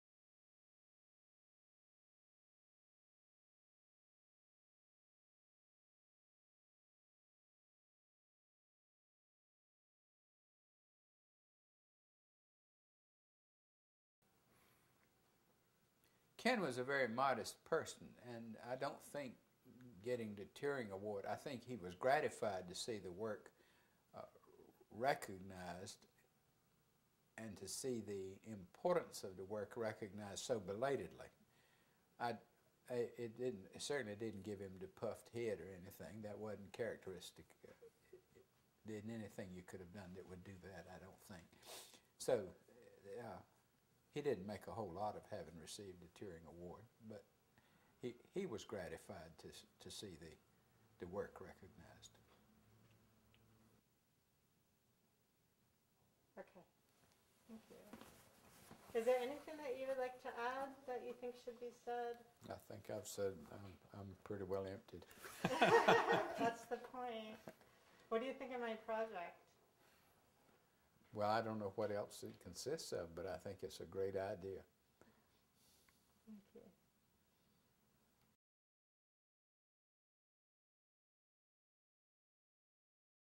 Interview with Dr Fred Brooks (Excerpt Oct 31, 2011)
In October of 2011, Dr Fred Brooks agreed to talk to me about when he and Kenneth E Iverson, the father of APL, shared an office at Harvard University in the 1950's.

Professor Dr Jan Prins of the University of North Carolina assisted with the interview.